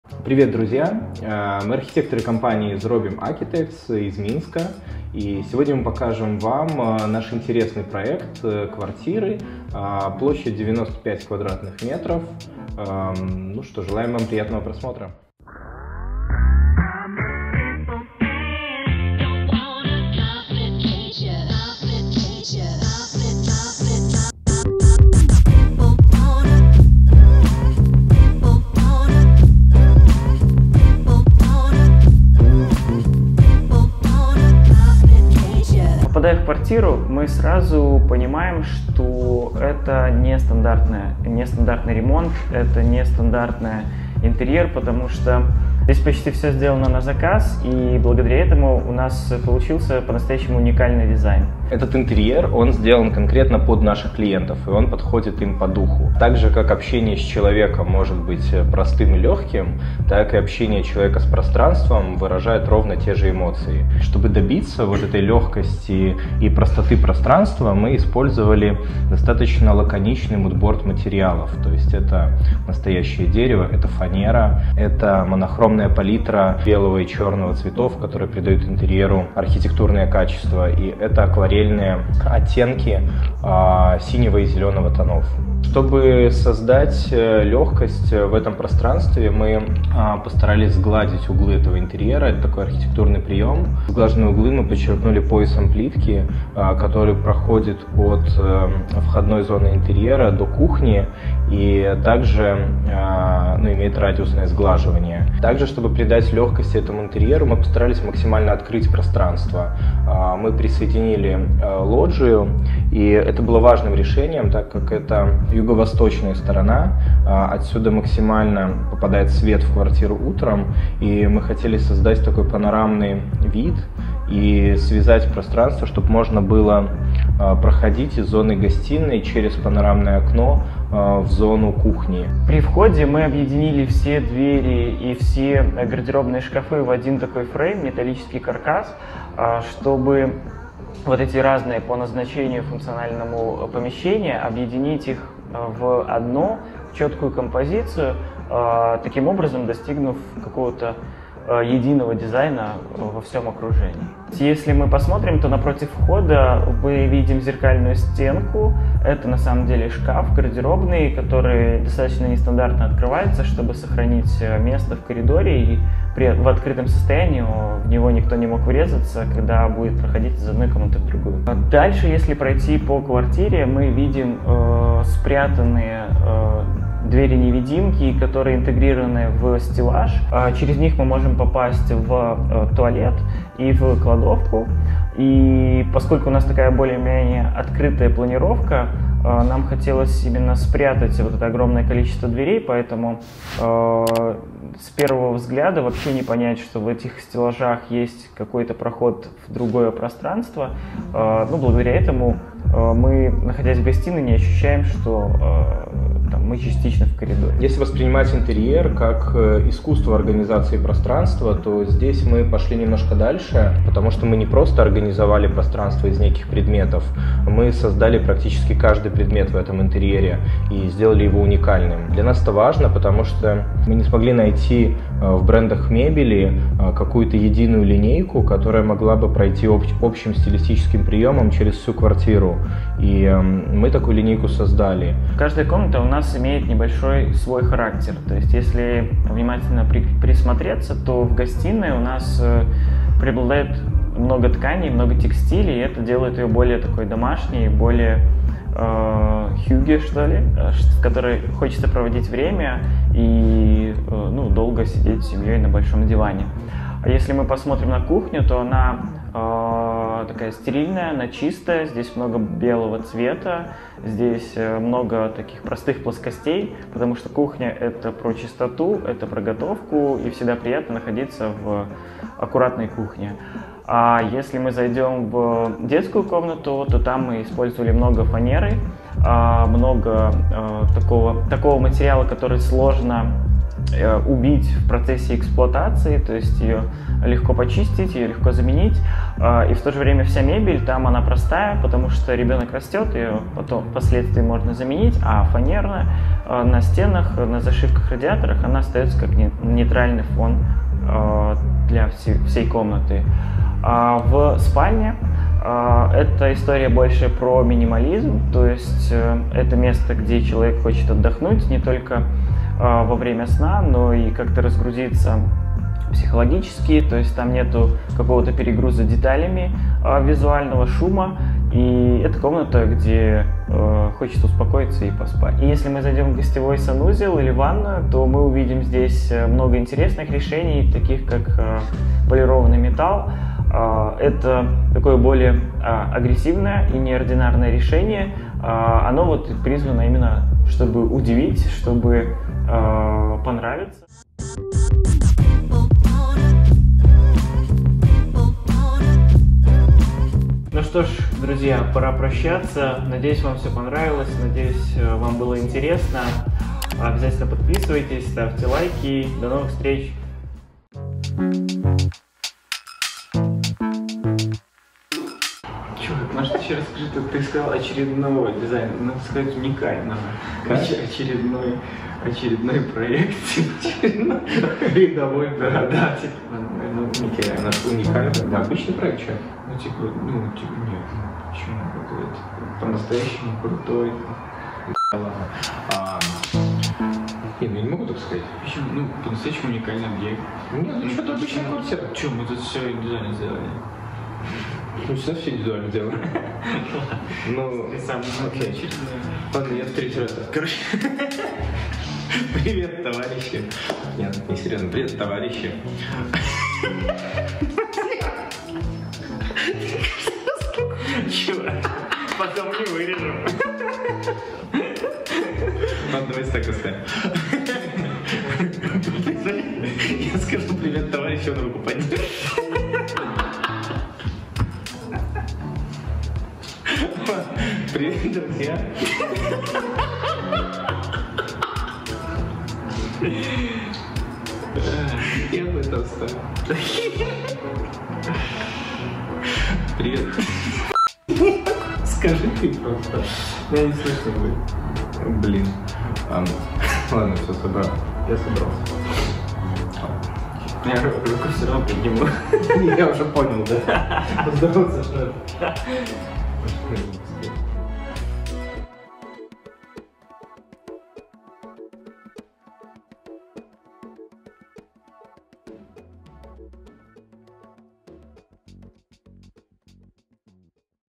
Обзор современной квартиры в стиле минимализм, дизайн квартиры 95 м2 для молодой семьи
Дизайн интерьера современной квартиры в стиле минимализм в Минске для молодой семьи с детьми. Обзор квартиры площадью 95 кв.м.
Изначально в квартире не было перегородок, благодаря чему наши архитекторы смогли учесть все пожелания Заказчика. Таким образом, мы смогли спроектировать квартиру, в которой есть большая гостиная, совмещенная с кухней, детская комната, 2 санузла, спальня с гардеробом и кладовая. Также мы объединили кухню и гостиную с небольшим балконом.  Обо всем этом расскажут руководители студии ZROBYM architects — Андрусь Bezdar и Алексей Кораблёв.